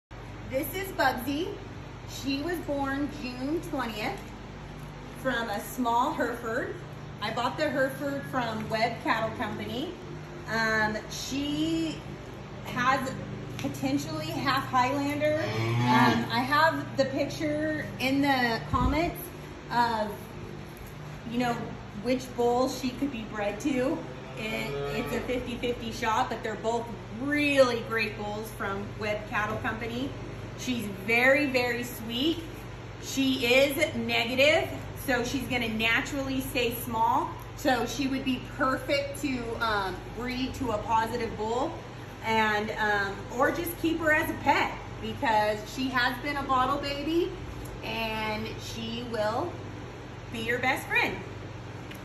Bugsy, the Hereford/Highlander Cross Heifer
BUGSY Is A Bottle Fed Heifer With A Sweetheart Personality And Adorable Looks To Match. She Comes From A Naturally Small Hereford And A Small Highlander Bull. This Calf Is By Far A Barn Favorite With Looks That Are Over The Top And The Best Personality. Bugsy's Parents Came from Webb Cattle Company In Missouri.  Potential Sires Can Be Viewed In The Photos And Are Credit Of Webb Cattle Company. Chrondo Negative, Does Not Have The Dexter BD1 Bulldog Mutation.